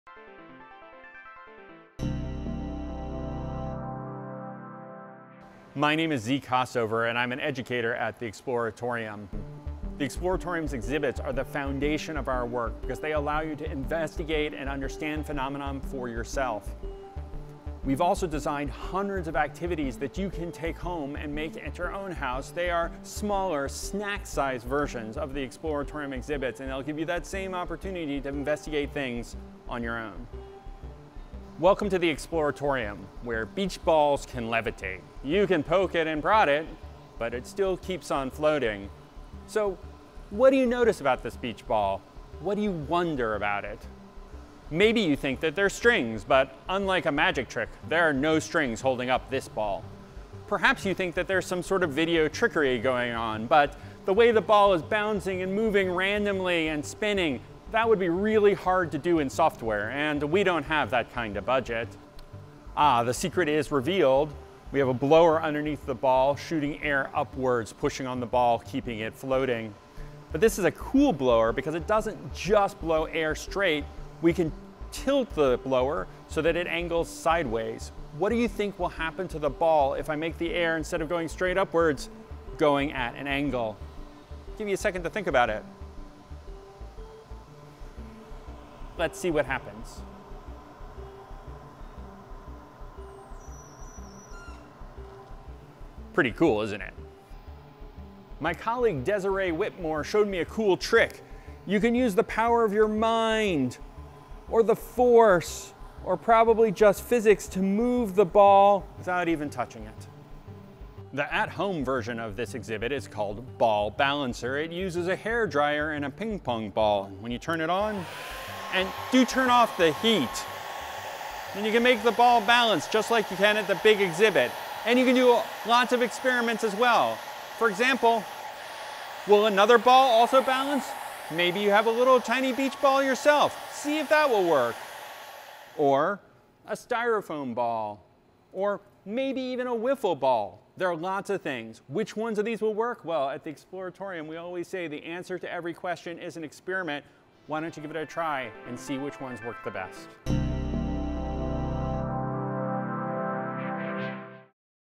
Balancing Ball | Build Your Own Exploratorium
A beach ball floating mysteriously in a stream of air, Balancing Ball enthralls curious children and adults alike. One of the most-visited exhibits at the Exploratorium, it explores the complicated ways that an invisible stream of air can interact with solid objects—like planes and birds—to make them fly. Join Staff Educator Zeke Kossover in exploring this exhibit and see how to make a levitating ball with a hair dryer.